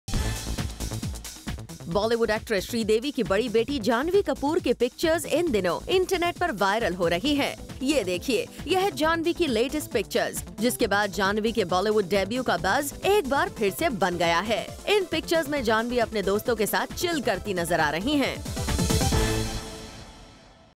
H0T! Sridevi's Daughter Jahnavi Kapoor SPOTTED Partying With Her Friends | Bollywood News
Sridevi's Daughter Jahnavi Kapoor's pictures are going viral on the internet.